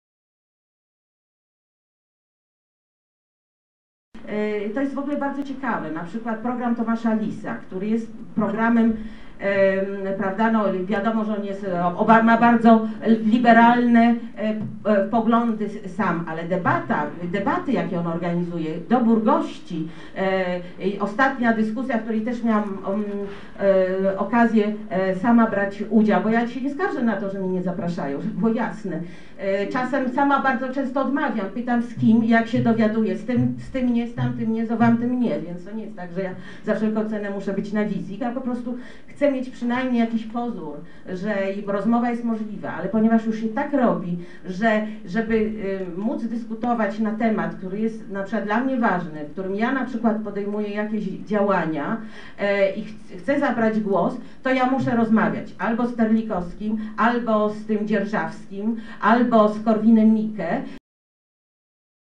Wanda Nowicka o uczestnictwie w programach publicystycznych
Wanda Nowicka ubolewa, że musi rozmawiać z Korwinem-Mikke, Dzierżawskim i Terlikowskim - 28 września 2014 r., Dni Świeckości, Kraków - Nagranie Fundacji Pro-Prawo do Życia